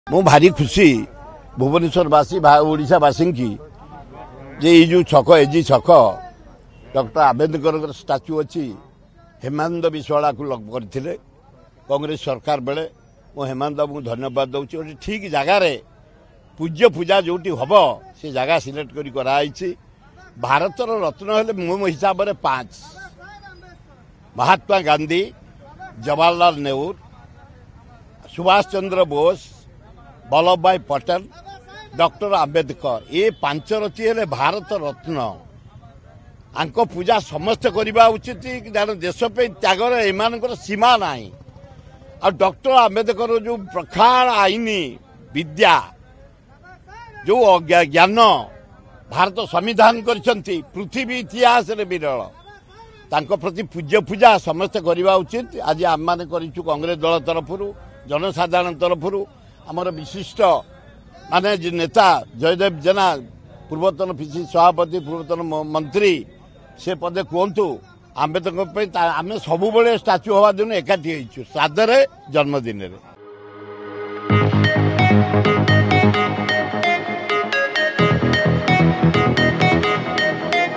Congress Leader Sura Rout Ray Reaction On Ambedkar Jayanti | Odisha Reporter
ଆଜି ବି ଆର ଆମ୍ବେଦକର ଜୟନ୍ତି । 130 ତମ ଜୟନ୍ତୀ ଅବସର ଭୁବନେଶ୍ୱର ଏଜି ଛକ ସ୍ଥିତ ଆମ୍ବେଦକରଙ୍କ ପ୍ରତିମୂର୍ତ୍ତୀରେ  ମାଲ୍ୟାର୍ପଣ କରିଛନ୍ତି ବିଭିନ୍ନ ରାଜନୈତିକ ଦଳର ନେତା କର୍ମୀଙ୍କ ସମେତ  ବିଭିନ୍ନ ଦଳିତ ସଂଘ  । ଆଜି ଦିନ ସମସ୍ତେ ବାବା ସାହେବ କୁ ମନେ ପକାଇଛନ୍ତି।